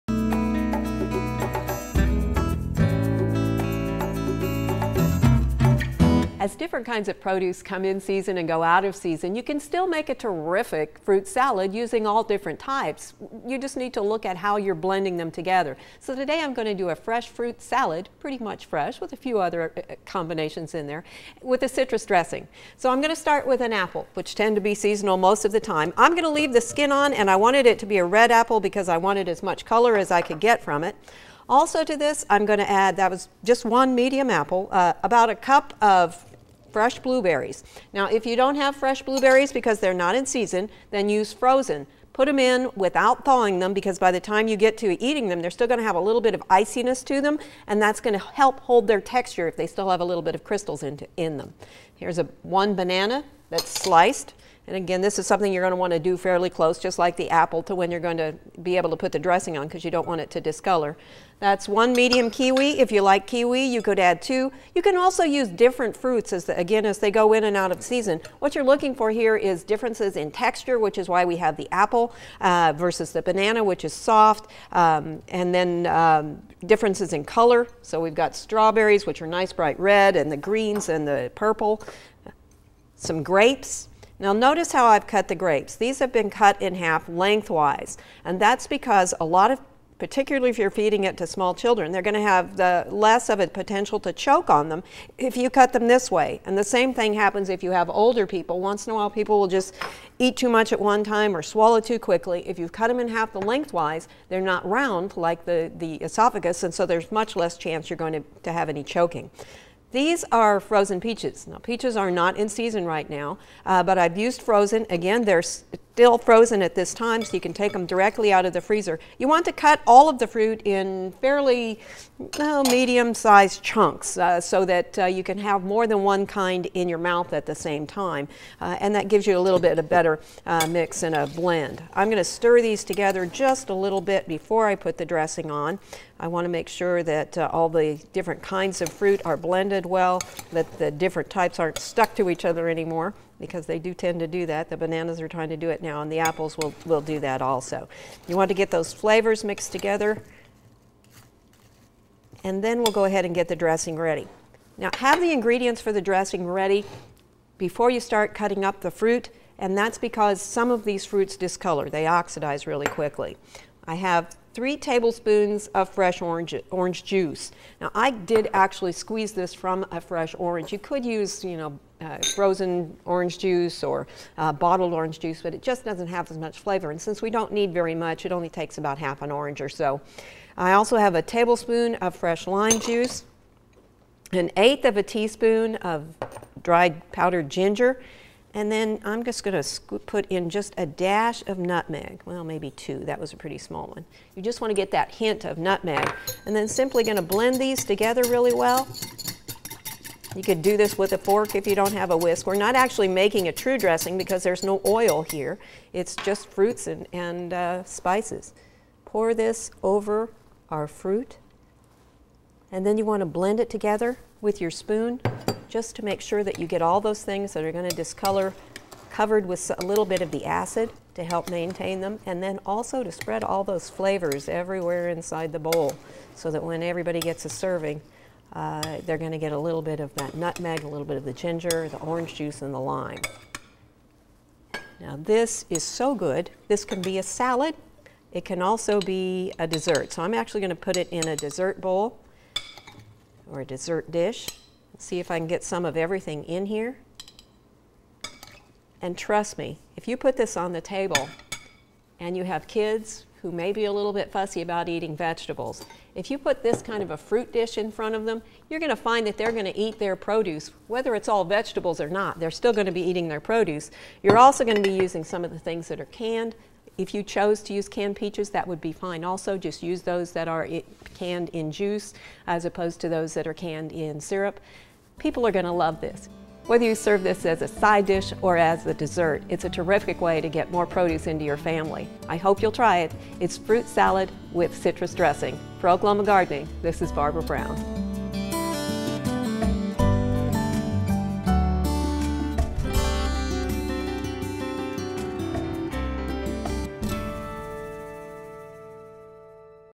Fresh Fruit Salad with Citrus Dressing YouTube sharing
Give your fruit salad a tangy flavor by making a homemade citrus dressing to mix in.